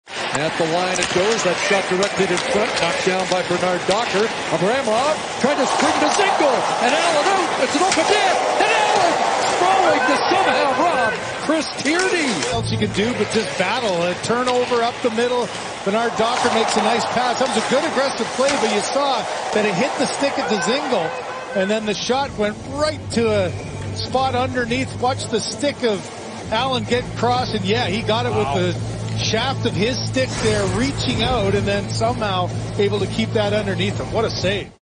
Jake Allen pokes the puck away, then dives back for miraculous save
Jake Allen breaks up the Dzingel breakway, and immediately follows up with a diving save to prevent a sure goal against the Ottawa Senators